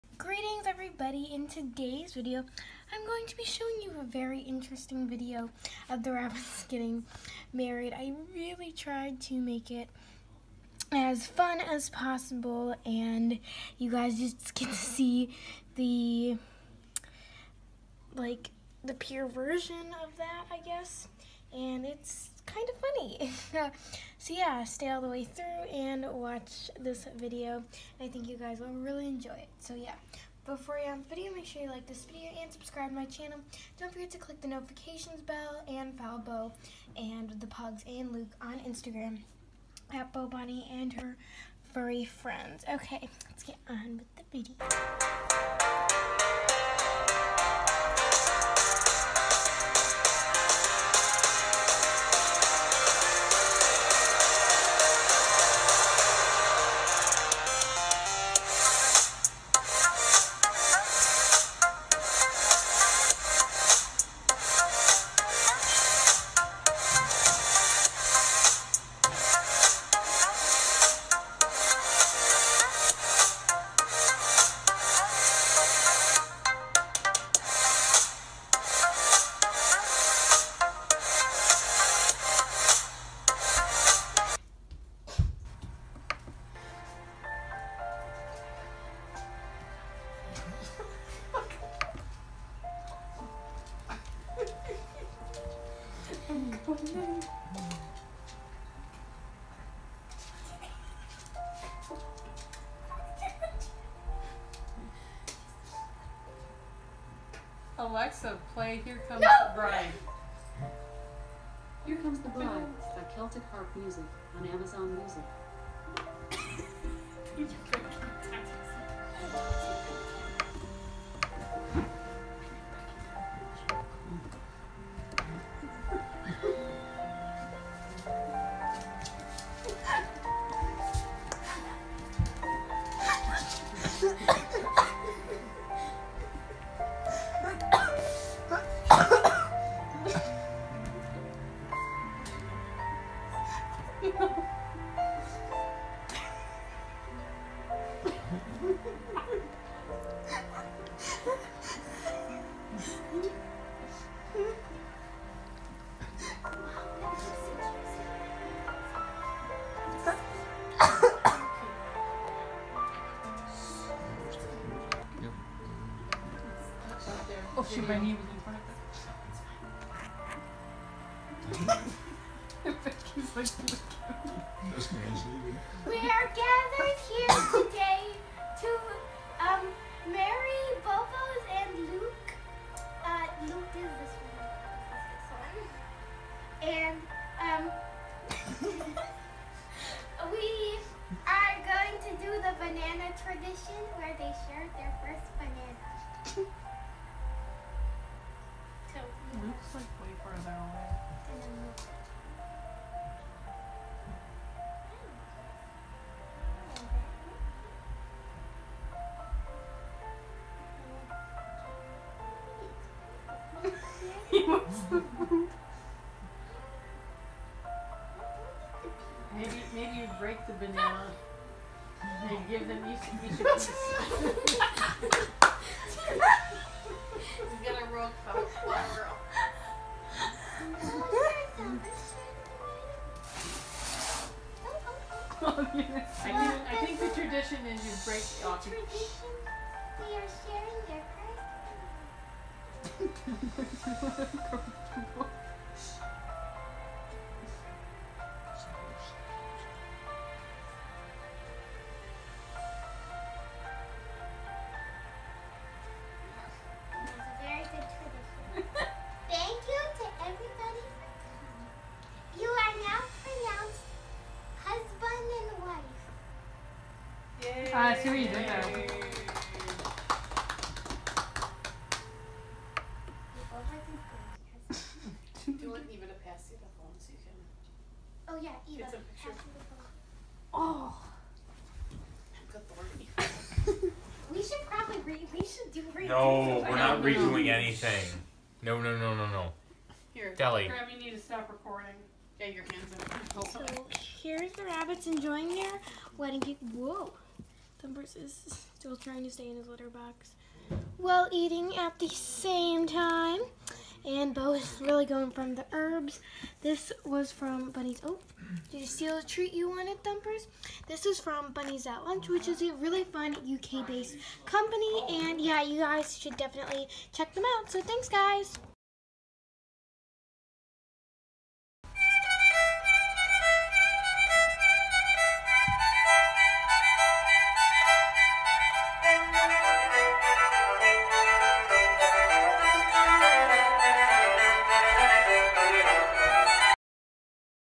The rabbit’s wedding
In today’s video I show you the rabbits wedding. The wedding part was totally unedited. I think it’s kinda funny so enjoy! Also don’t forget to click that little bell that way you get notified every time I upload. Follow Bo on Instagram; @bobunnyndherfurryfriends